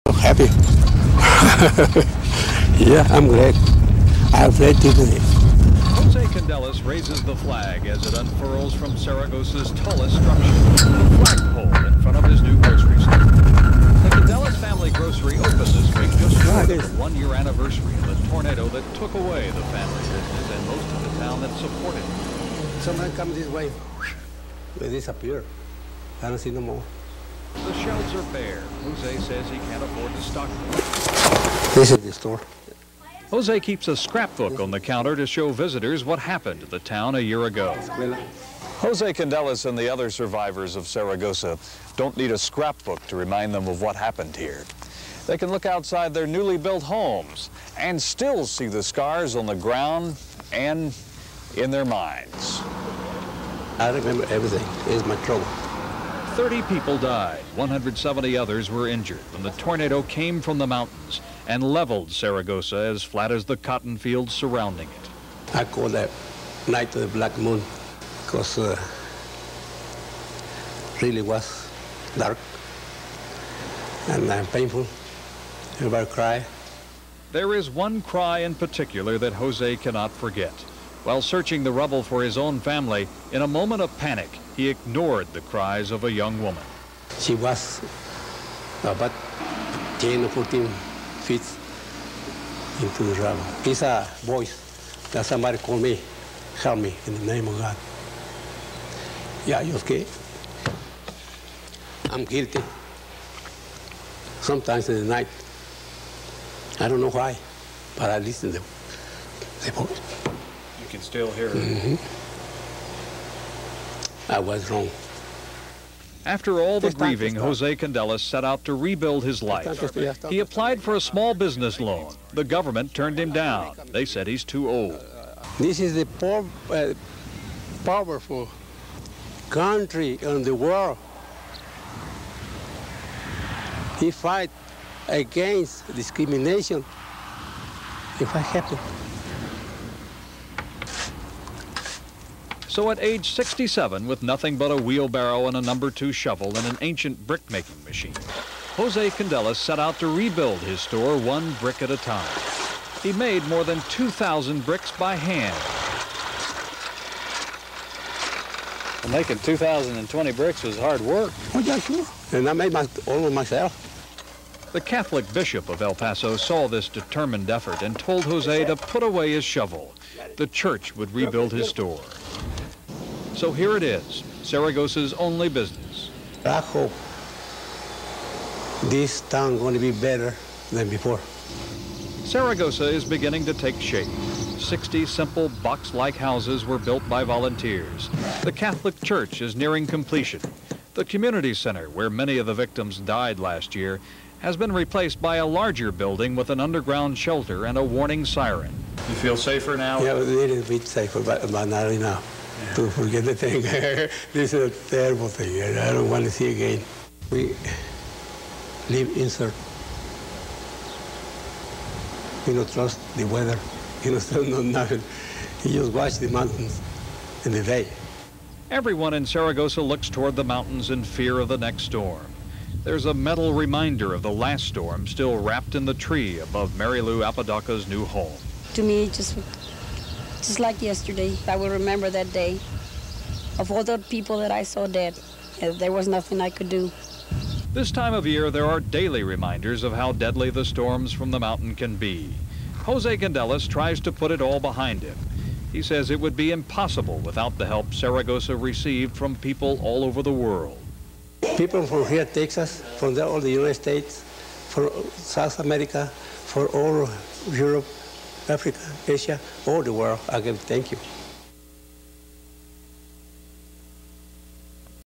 Saragosa Tornado - 1 Year Later
A store owner lives with a haunting memory of a killer tornado in Saragosa, Texas. (1987)